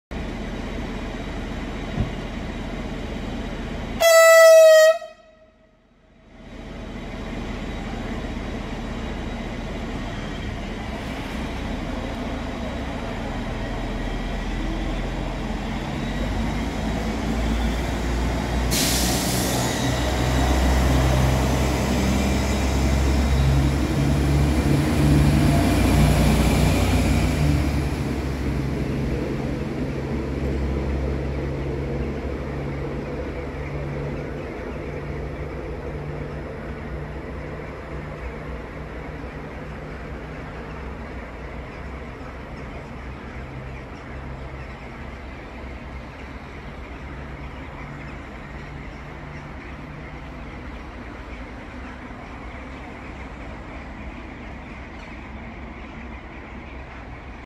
Mediaş: R 2567 train (Sibiu - Sighişoara). DMU nr. 96 2506-5
Mediaş: tren R 2567 (Sibiu - Sighişoara). Automotor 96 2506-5.